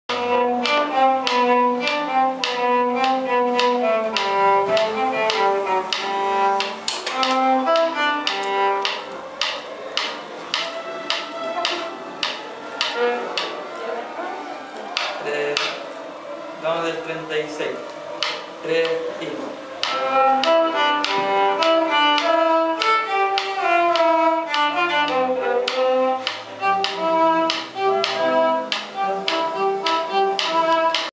Taller Violas orquesta B. Profesores Javier Huenchullan y Charles André Espindola
XIV Encuentro Internacional
Jóvenes Músicos
Enero 2011